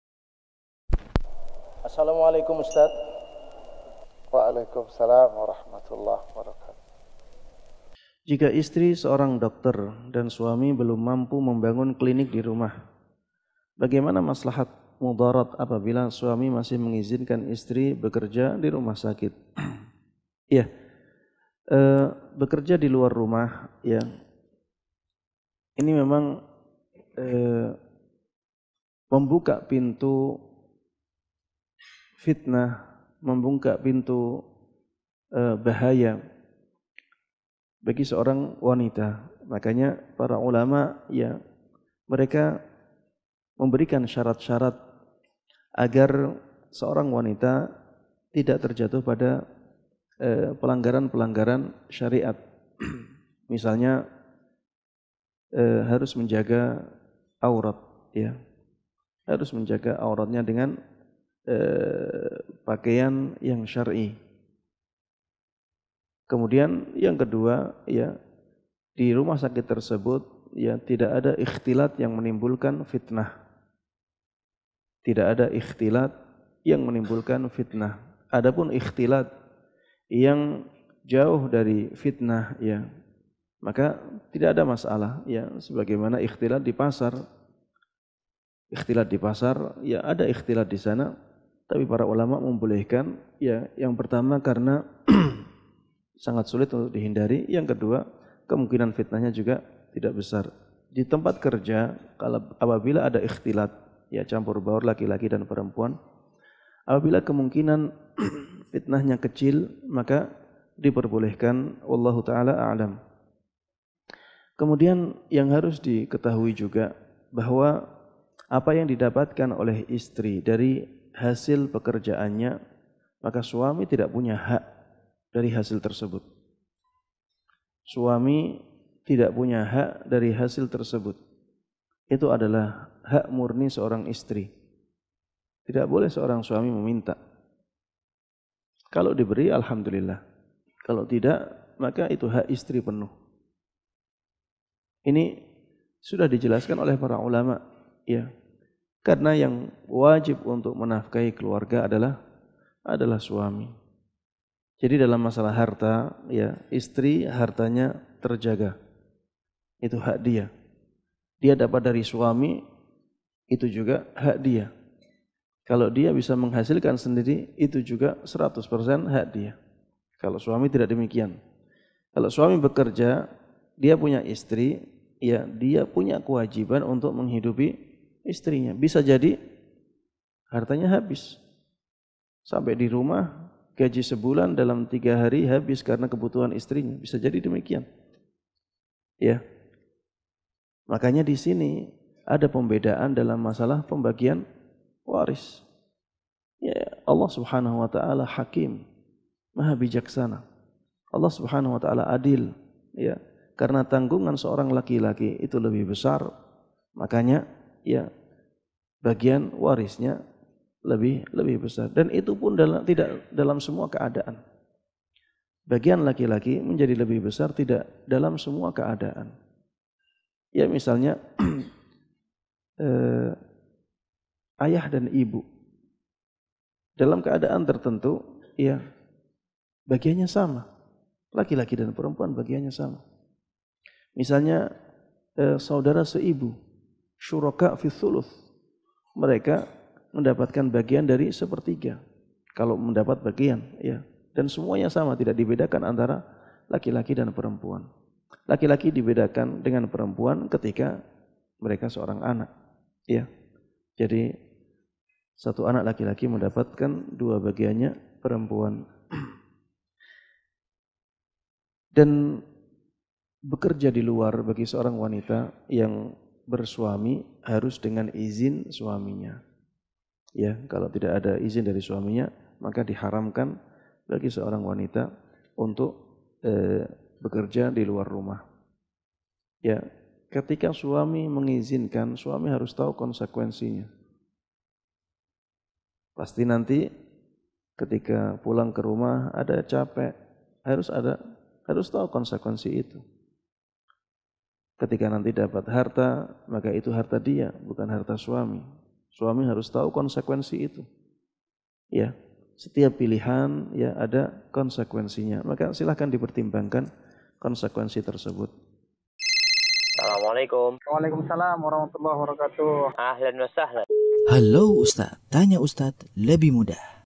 Solusi Istri Bekerja Seorang Dokter ? : Dr. Musyaffa Ad Dariny, MA
Halo Ustadz
Ustadz Menjawab

Dr. Musyaffa Ad Dariny, MA
Solusi Istri Bekerja Seorang Dokter ?

Download aplikasinya di Playstore
Tanya Ustadz Lebih Mudah

Barakallohu fiikum
Selamat Menyaksikan